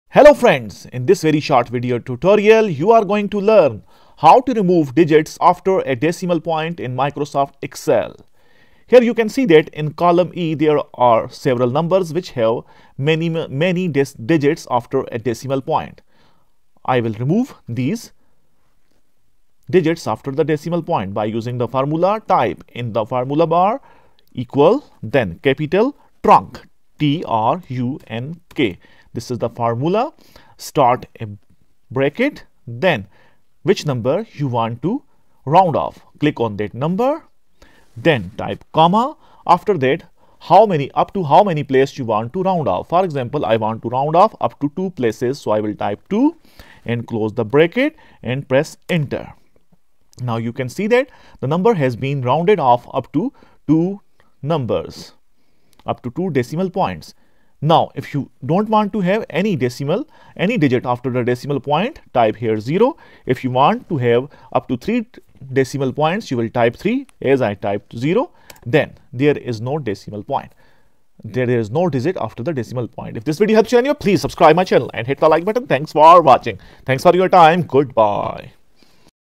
How To Remove Digits After Decimal Point In MS Excel | Round off Number in Excel | Round off Formula
This tutorial of Microsoft Excel shows how to round off numbers in Microsoft Excel. After watching this video you will be able to remove digits after the decimal point in Microsoft Excel with the help of round off formula.